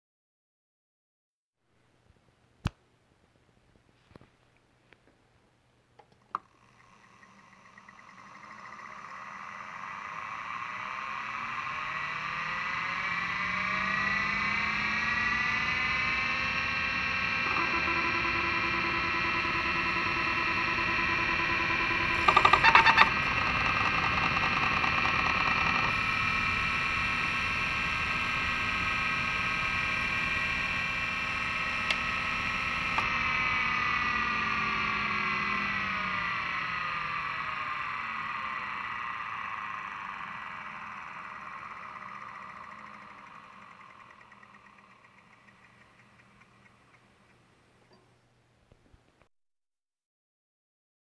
Early 80's 5mb Hard Drive operating/seeking sound
I LOVE the sound of old hard drives and mechanical hard drives in general, so I thought I'd share it on the internet :P